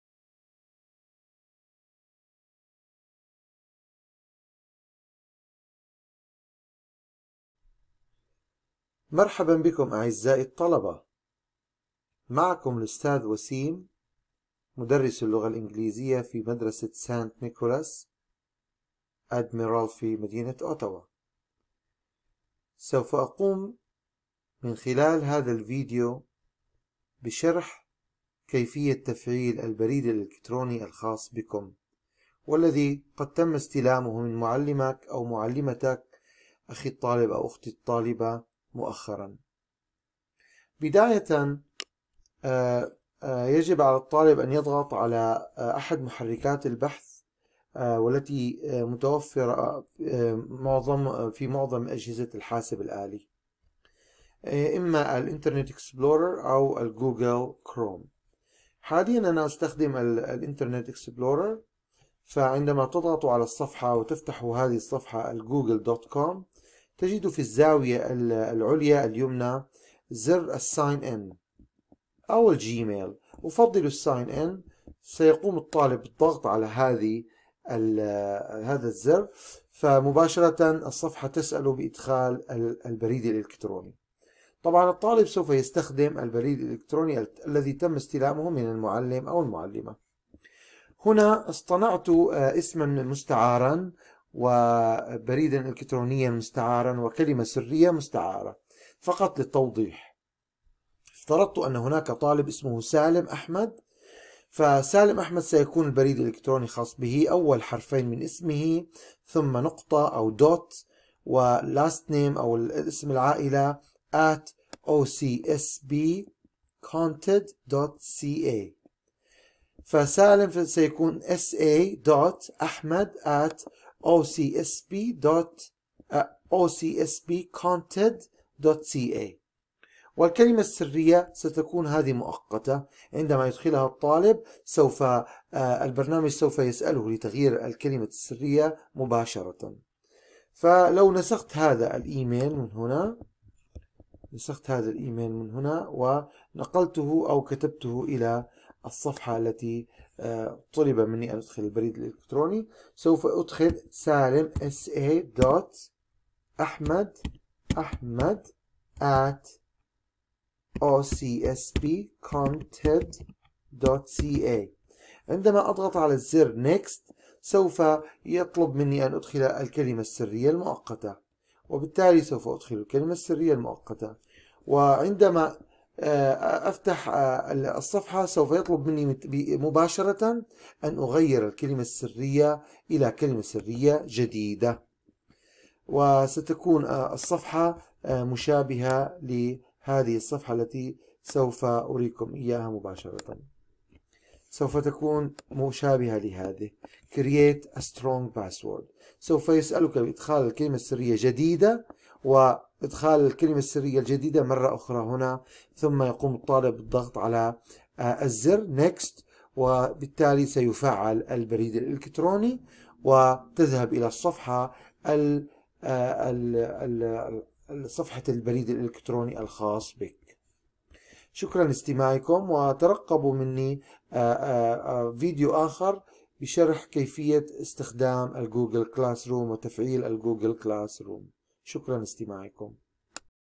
ocsbHowTo Activate Your OCSB Gmail For Arabic Speaking Learners
Instructional video on how to activate your OCSB Gmail account for Arabic Speaking Learners from OCSB Continuing & Community Education